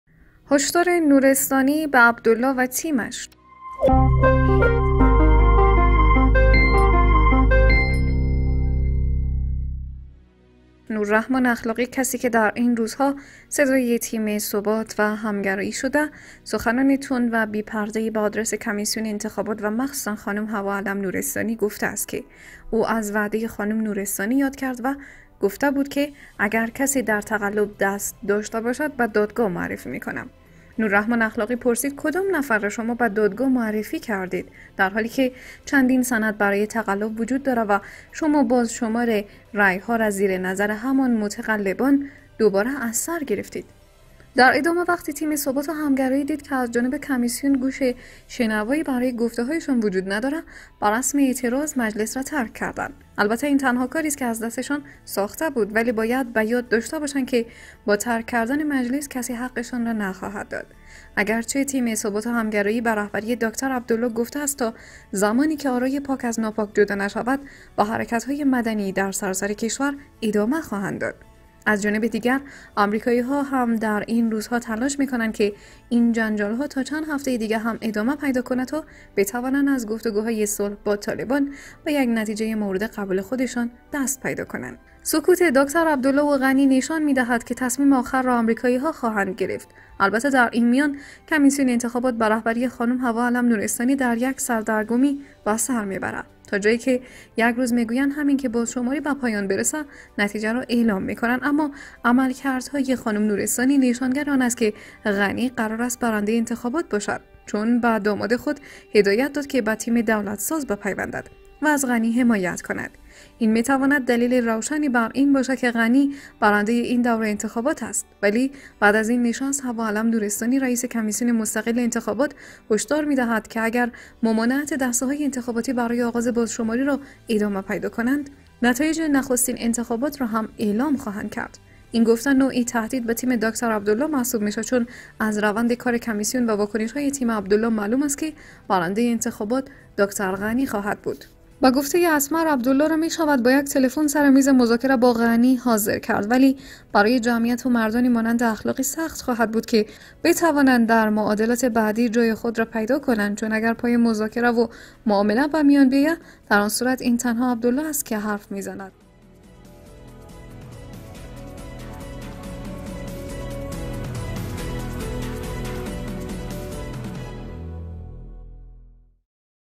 Parasto TV|هشدار نورستانی به عبدالله و تیم اش
A conference that had a lot to say, but it didn't work out